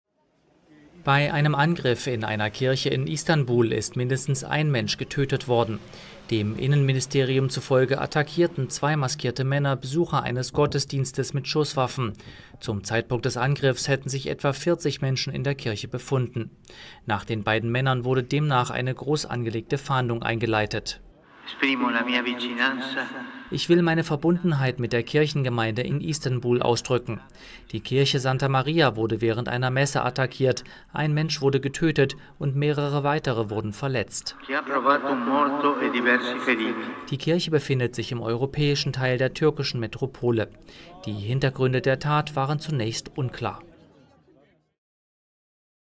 Istanbul: Ein Toter bei Angriff in Kirche
Dem türkischen Innenministerium zufolge attackierten zwei maskierte Männer die Besucher eines katholischen Gottesdiensts mit Schusswaffen. Nach den Tätern wird gefahndet – die Hintergründe der Tat sind bislang unklar.